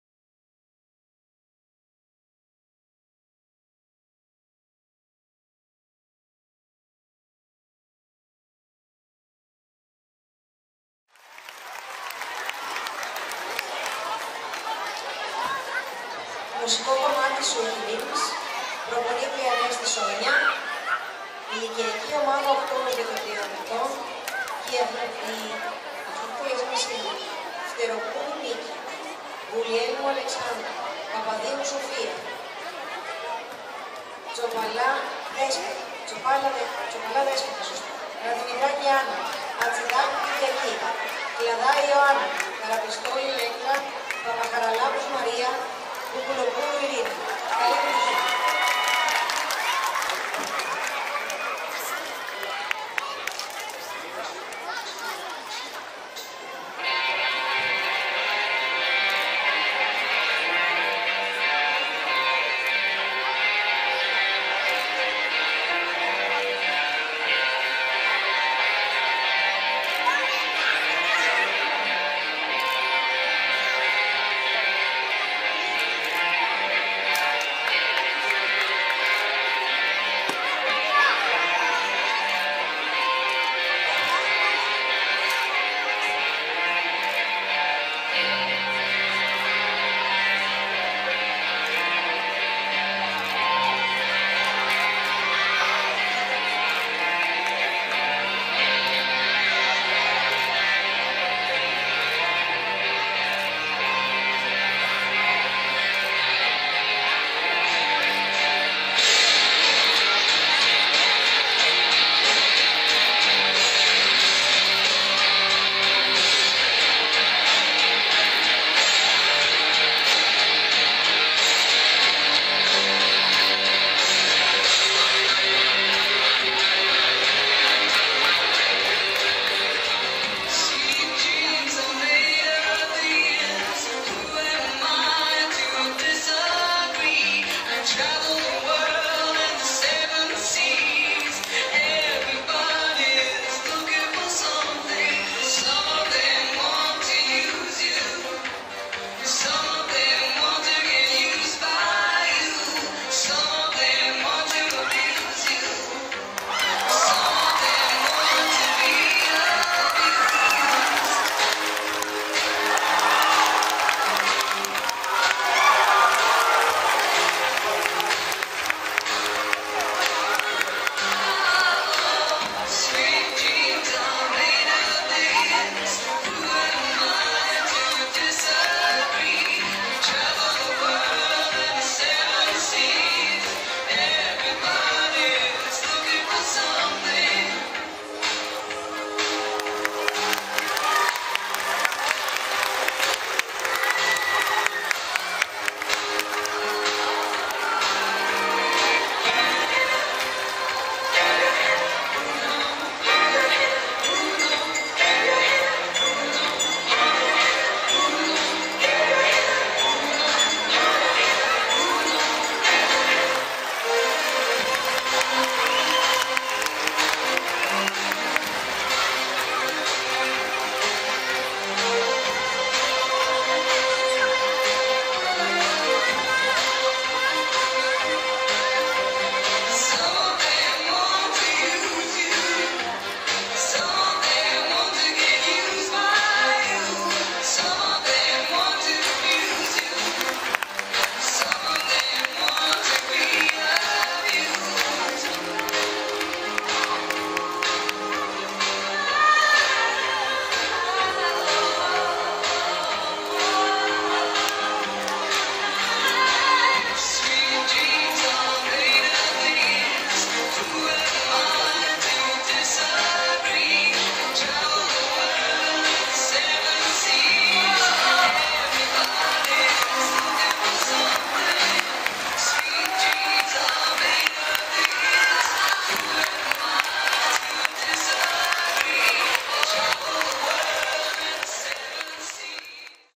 Γυμναστικές Επιδείξεις Γύμναθλος Ηλιούπολης 2022 - Sweet dreams
Γυμναστικές επιδείξεις του Γύμναθλου Ηλιούπολης για το έτος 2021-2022, στο κλειστό γυμναστήριο Ηλιουπόλεως. Επίδειξη ομάδας Sweet Dreams